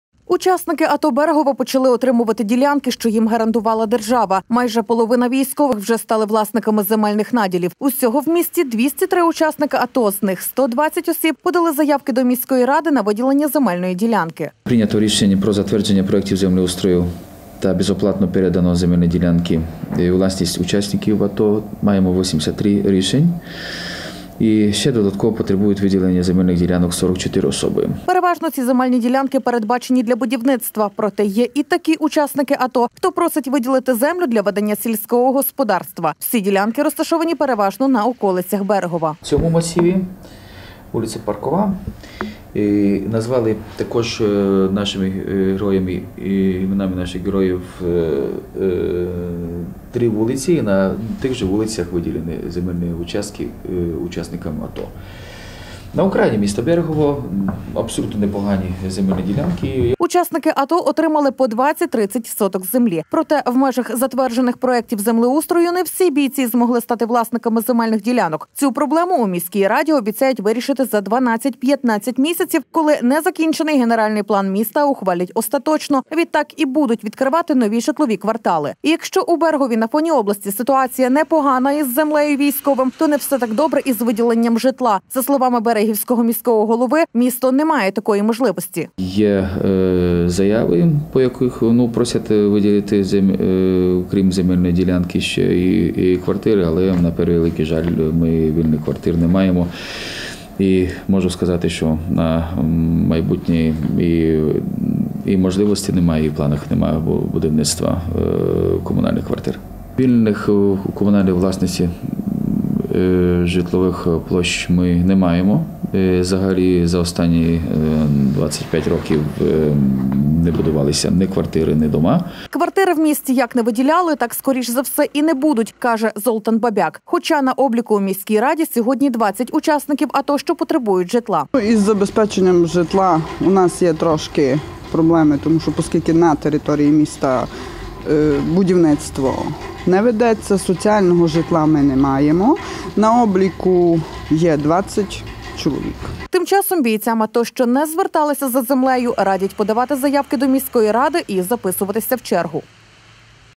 Учасники АТО Берегова почали отримувати ділянки
Законодавство України передбачає систему пільг для учасників АТО, в тому числі військові мають право на першочергове отримання земельних ділянок. Своїм правом скористалося вже 83 бійці Берегова.